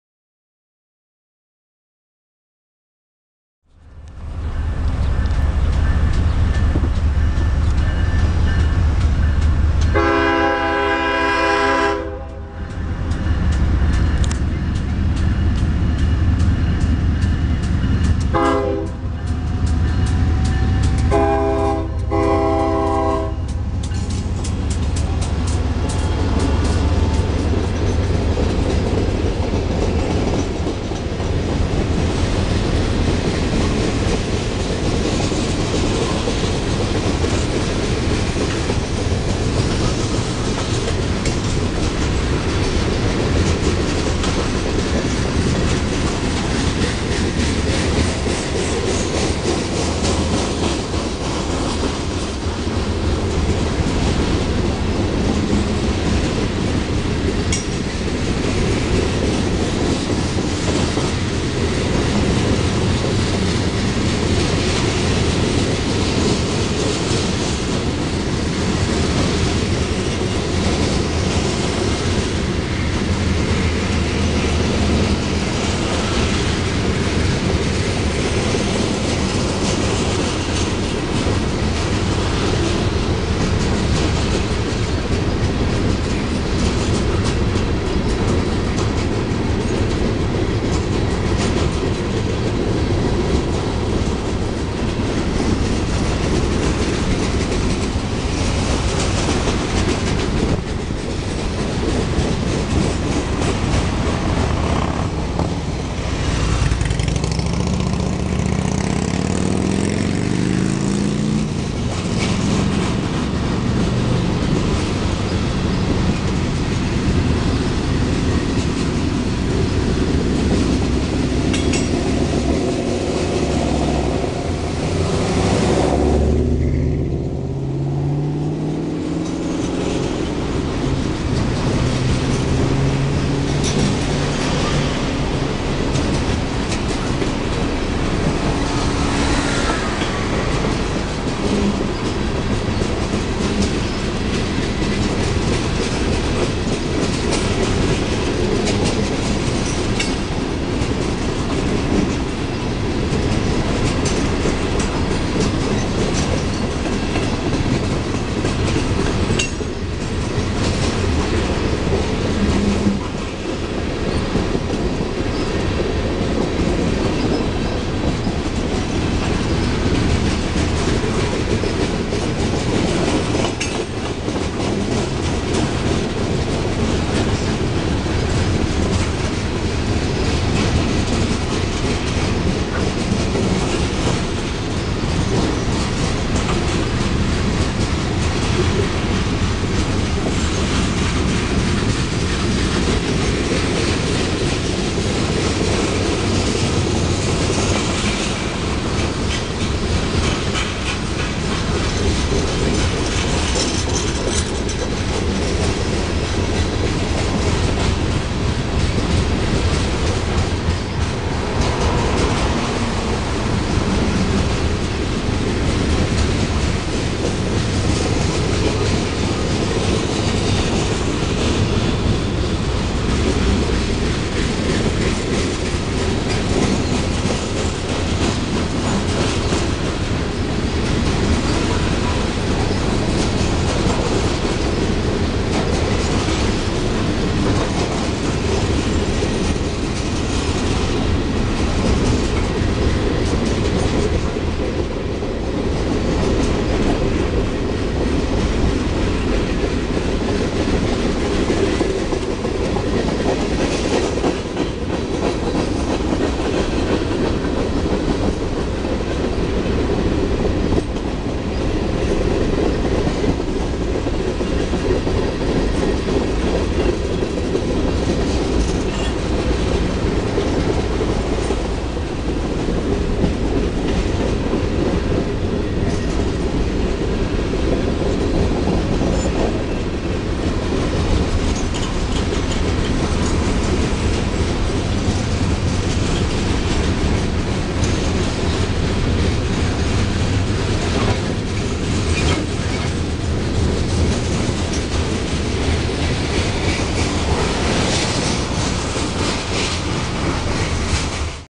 NS May 29, 2011
NS eastbound on the single track section of the Conemaugh Line in O'Hara Township, PA. Sunday, May 29, 2011.